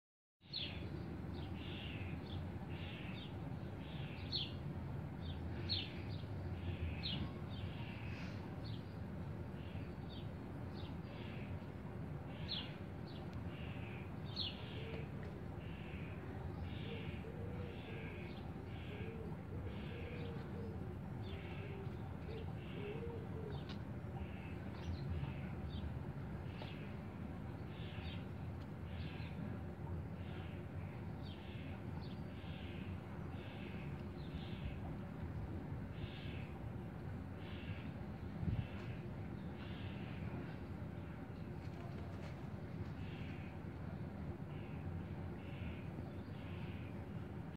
Campion playing
Grass needed time to regrow, so when he was finally allowed in, binkies ensued!